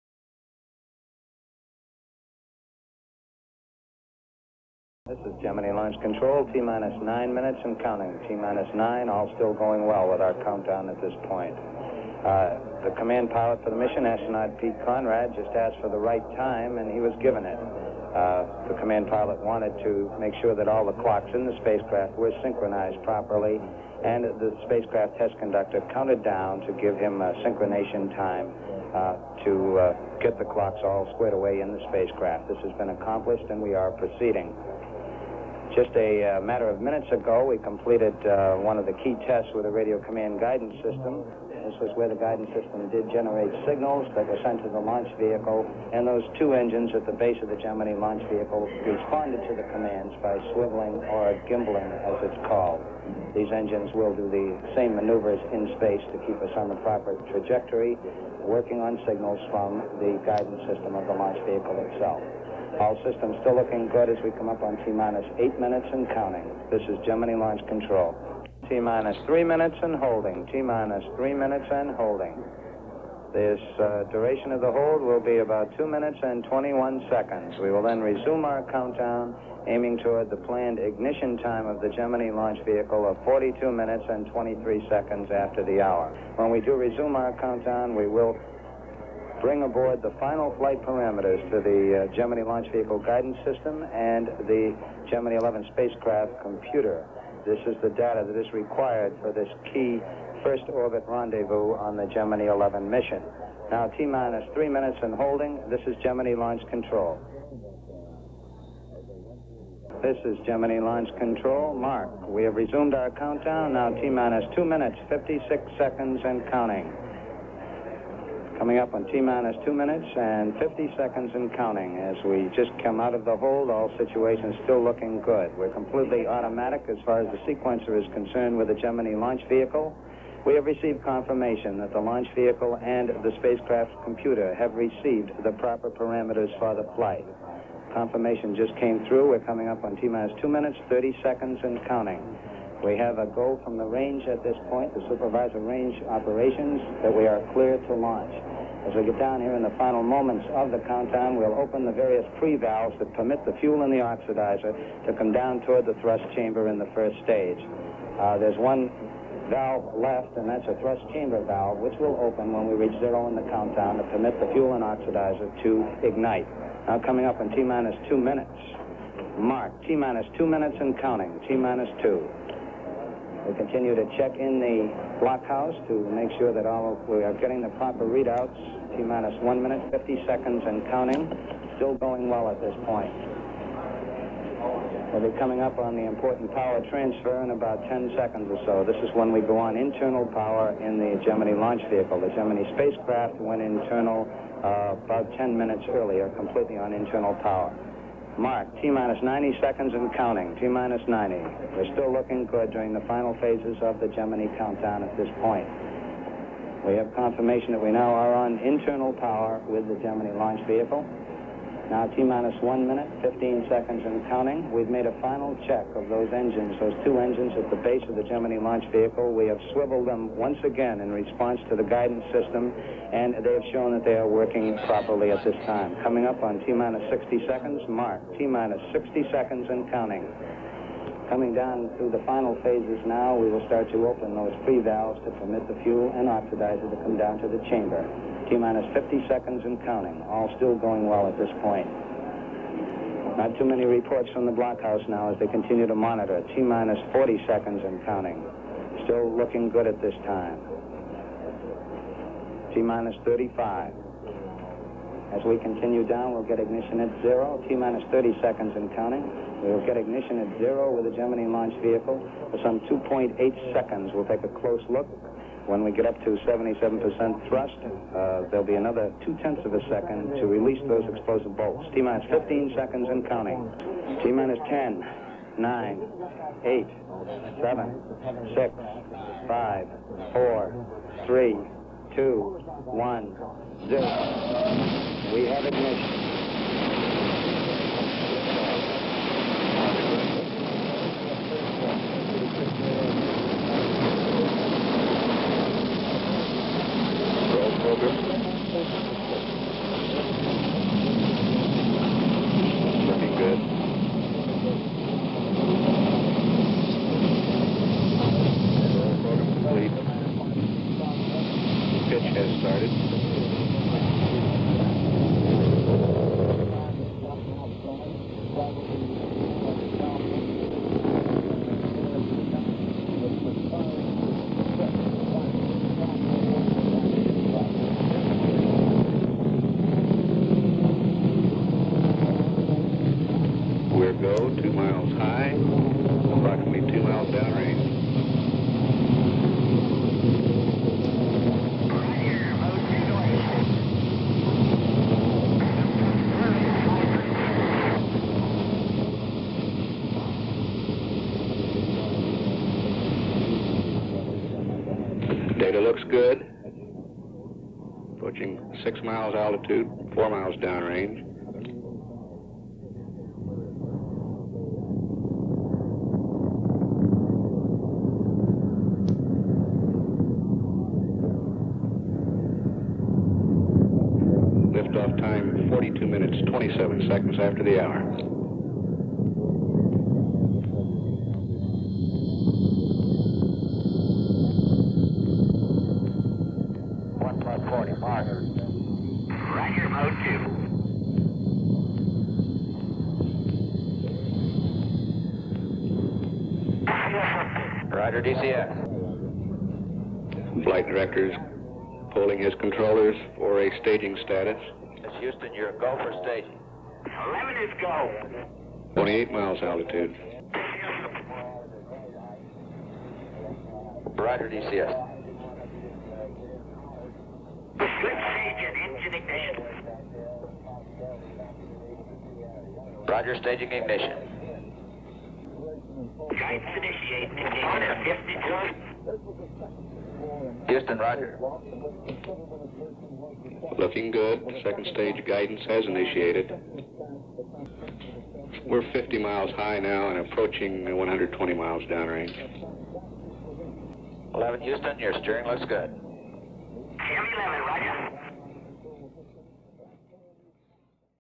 Gemini XI Launch | September 12 1966
On this edition of @mannedspace1187 we remember the launch of Gemini XI on September 12, 1966.  Gemini IX marked the return to space of astronaut Charles "Pete" Conrad who was in command of the mission.  He was joined by friend and fellow naval aviator astronaut Richard "Dick" Gordon.  This was Gordon's first space flight.

All black and white footage shown throughout this video is courtesy of Universal Newsreel otherwise, all color footage is courtesy of NASA.  Audio Original sound source is Apollo16UVC
Universal Newsreel footage of the Gemini II launch taken on January 19, 1965 is used for historical perspective.

Edited By: Mitch Rothman